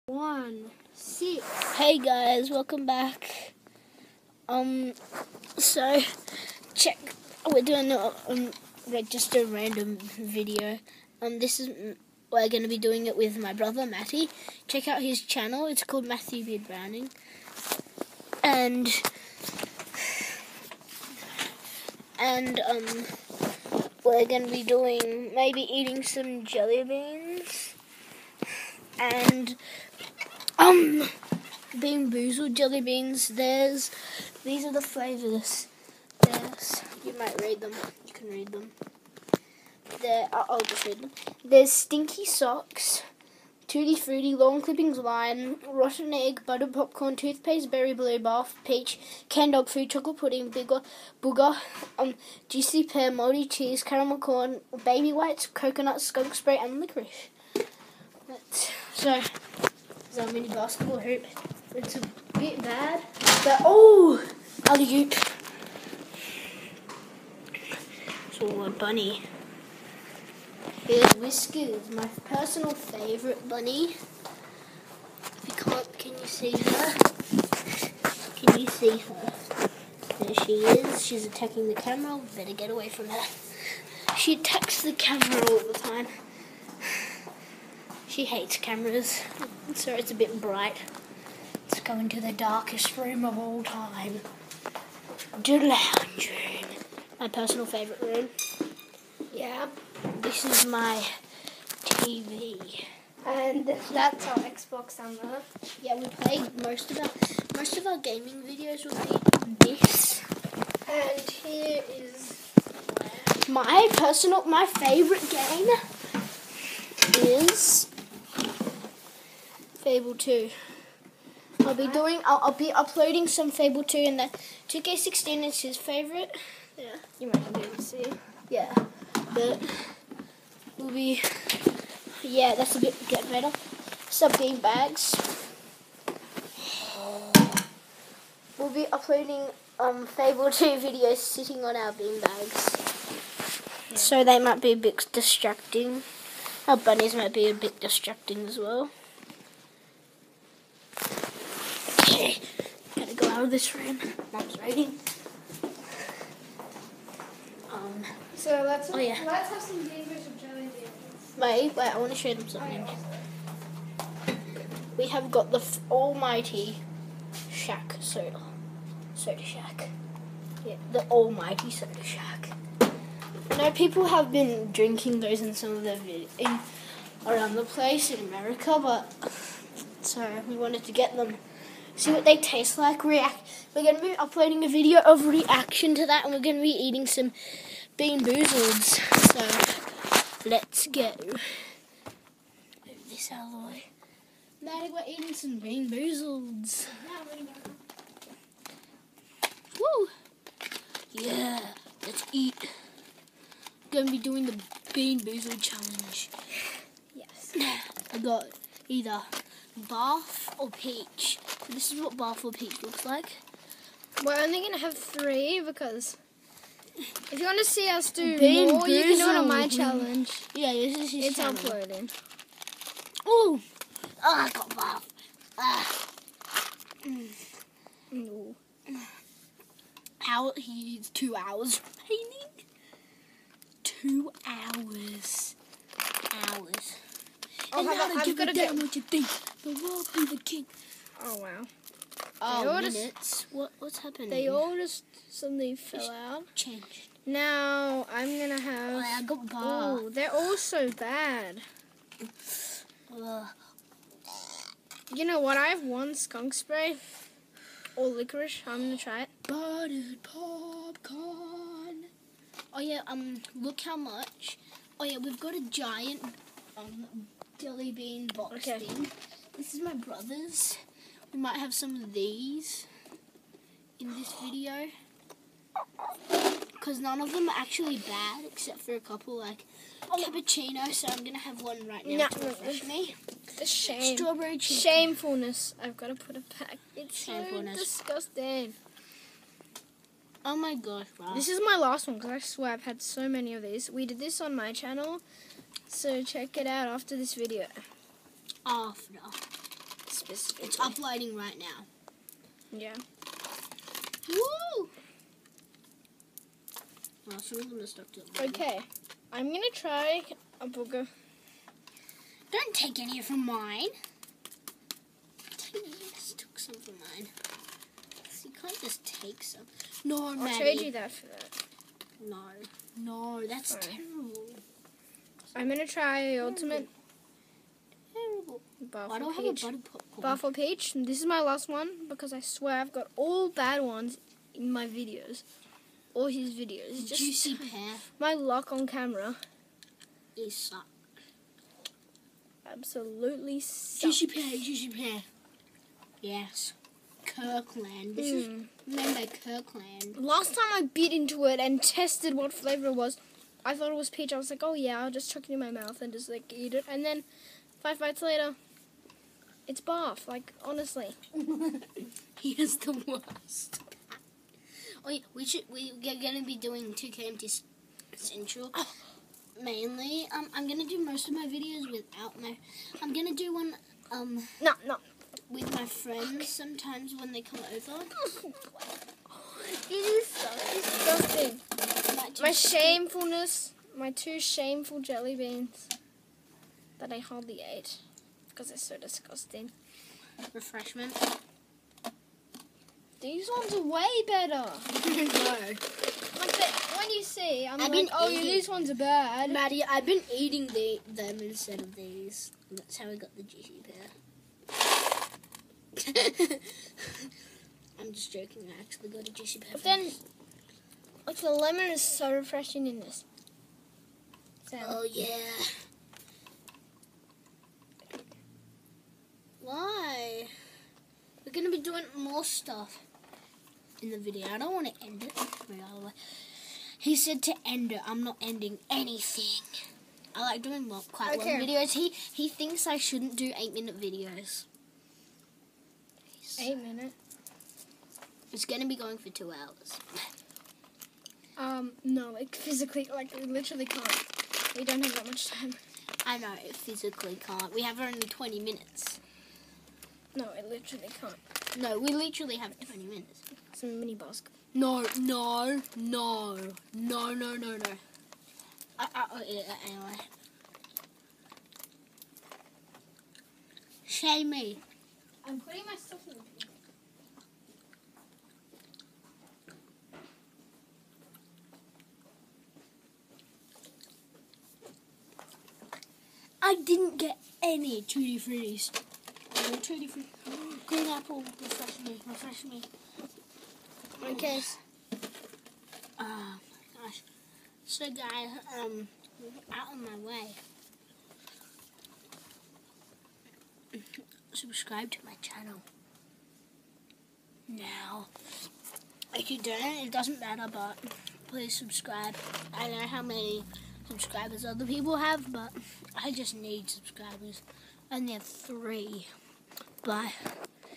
Random vlog + bean boozled challenge pull faces with brother
Check my bros channel called Matthew Beard-Browning